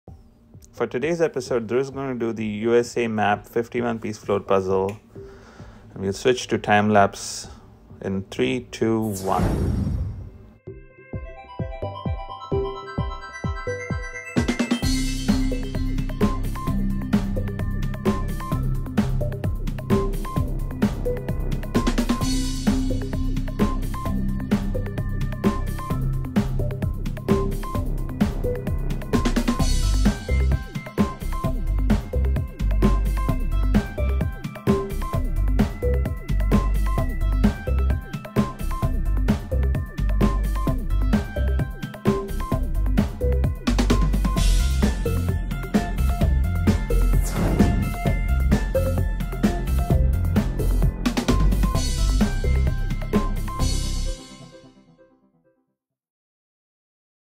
4 year old Dhruv builds the USA Map puzzle
Dhruv builds the Melissa and Doug 51 piece USA Map puzzle.